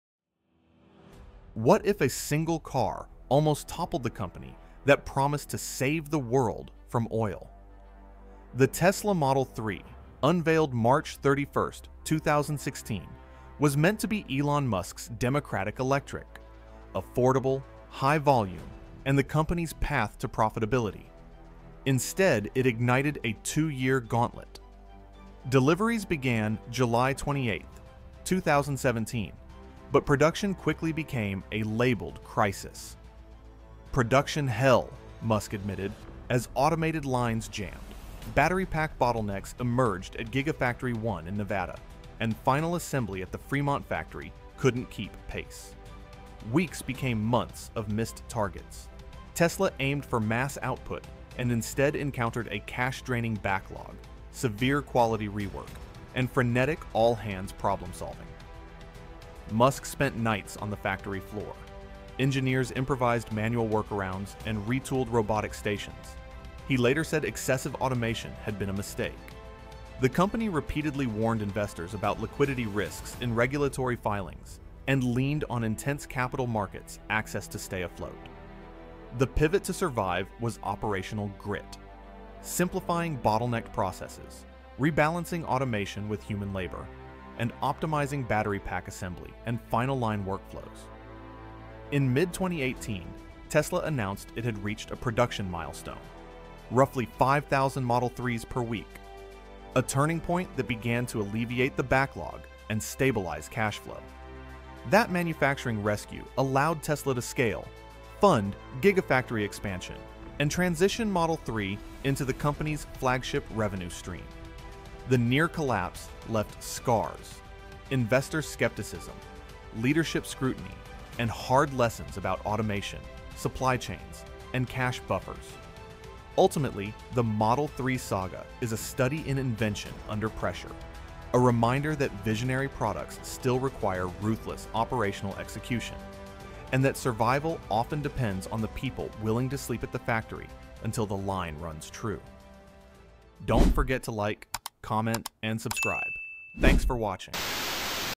The Electric Car That NEARLY Bankrupted Tesla!
What if one car almost broke the company that promised to end our dependence on oil? 🚗⚡️ This video breaks down the Model 3 saga — from the March 31, 2016 unveiling to July 28, 2017 deliveries, the “production hell” bottlenecks at Gigafactory 1 (Nevada) and Fremont, and the raw factory nights and process pivots that pushed Tesla to ~5,000 Model 3s/week in mid-2018. We’ll explore automation vs. human labor, battery-pack triage, investor panic, and the operational lessons that turned near-collapse into scale. Hit play for a fast, fact-first postmortem on one of modern manufacturing’s most dramatic turnarounds.